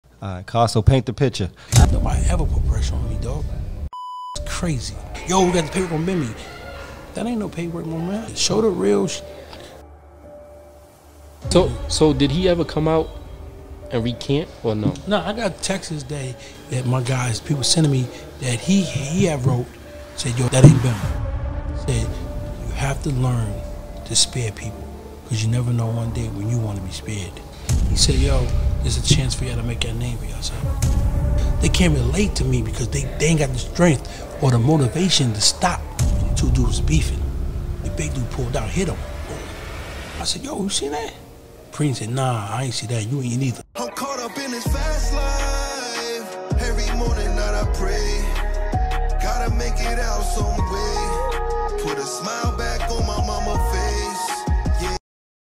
Next on the ghettoreportpodcast we have Queen’s finest, a Supreme Team legend
Next on the ghettoreportpodcast we have Queen’s finest, a Supreme Team legend @bimmy118. Dropping soon!!!